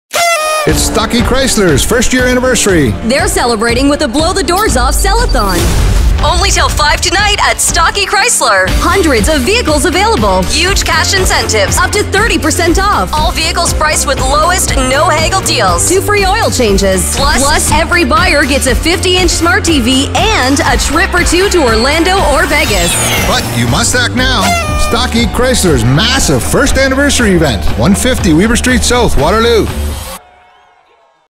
1st Anniversary Sale Ends Today!
For 3 days only - Thursday August 22nd, Friday August 23rd & Saturday August 24th - Stockie Chrysler Dodge Jeep Ram will be celebrating our 1st Anniversary with a massive, blow-the-doors-off sales extravaganza!